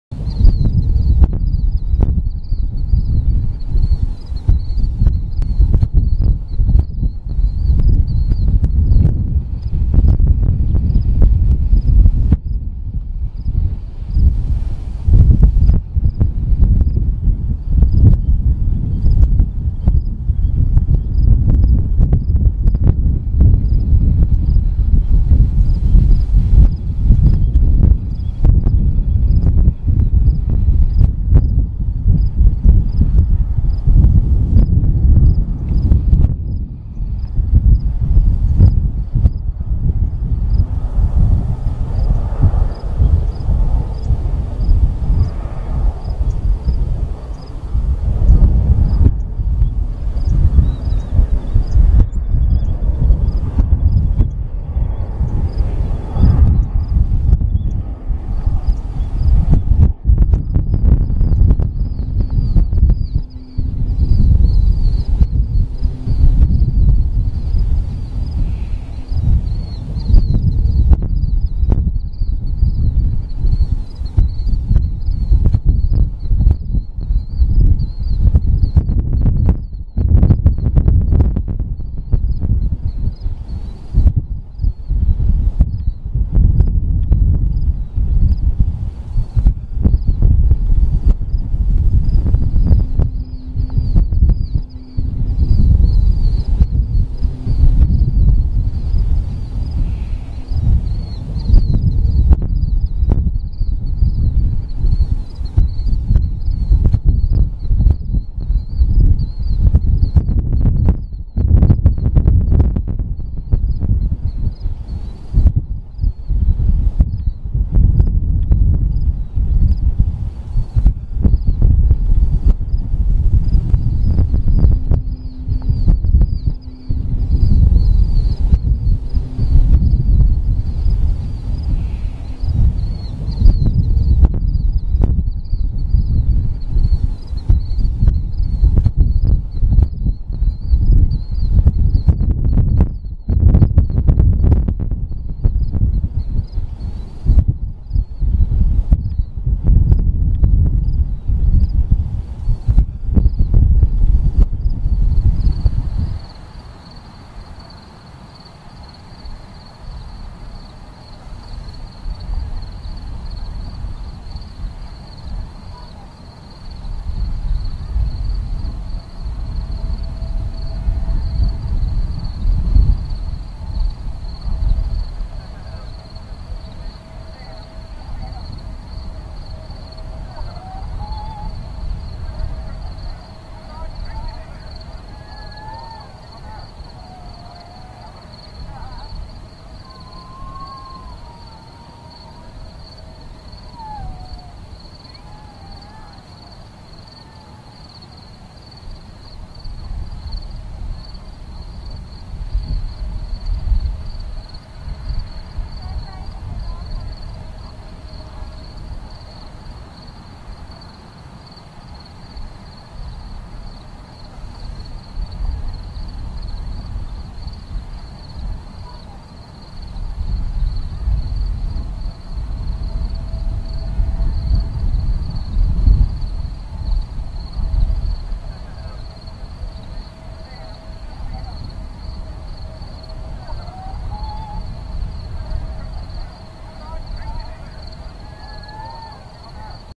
Kestrel in the Fraser river in Richmond BC - under smoke filled skies
Smoke from wildfires burning in Washington and Oregon USA are blanketing Vancouver and all my photos and video have a sepia tinge - visibility is really bad and the Air Quality index today is 200 - “Very unhealthy!” I was at Iona yesterday in Richmond BC and despite the awful conditions this lovely American kestrel was very cooperative and i really enjoyed filming him - hope you enjoy my video too - stay safe everyone!!